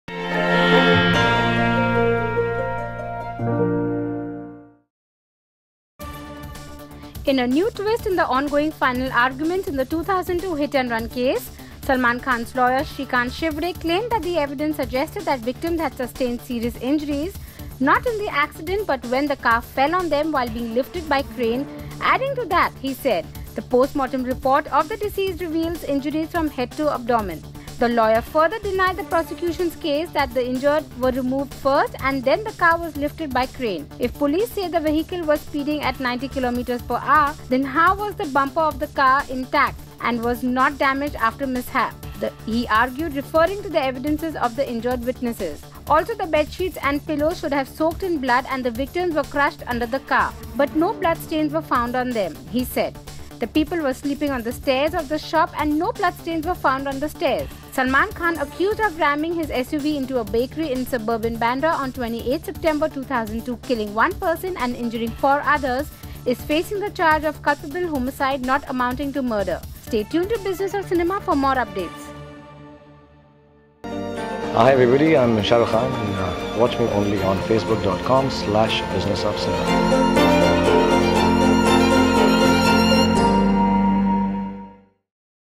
Hit And Run Case: Salman Khan's Lawyer Comes Up With A New Twist
Salman Khan’s 2002 hit and run case have come up with new twist and turns. Actor’s defence lawyer on Friday claimed that that the evidence suggested that victims had sustained serious injuries not in the accident. Watch video to know more.